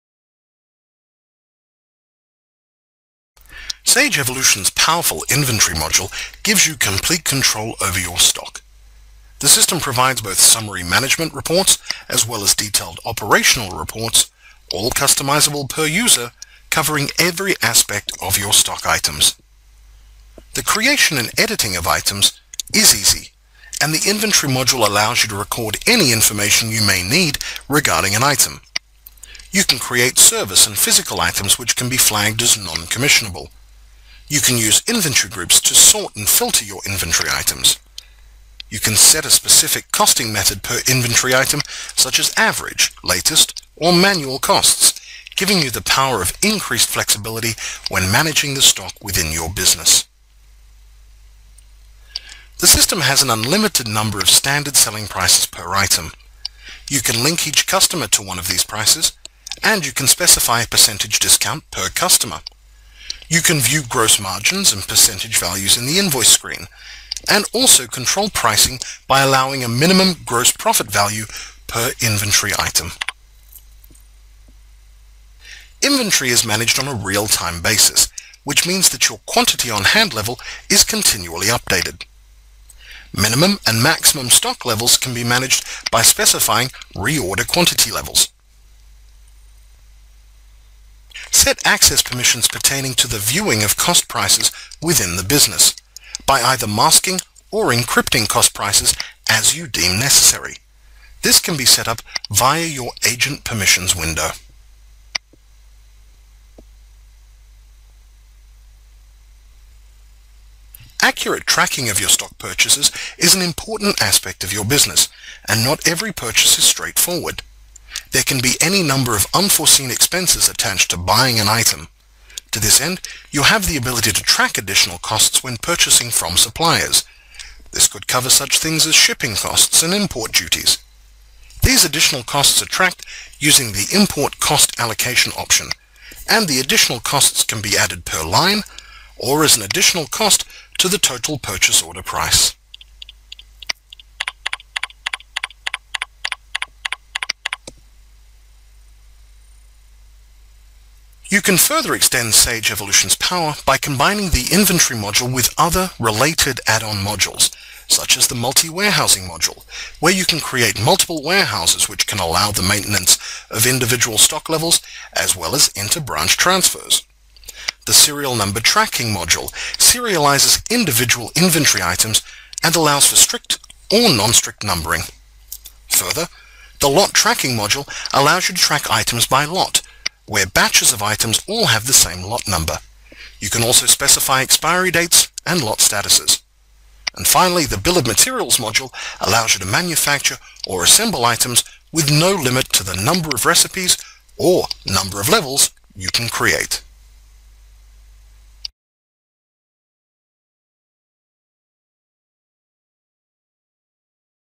1402 07 Sage Evolution Inventory
A quick overview of Sage Evolution's powerful and highly flexible Inventory module.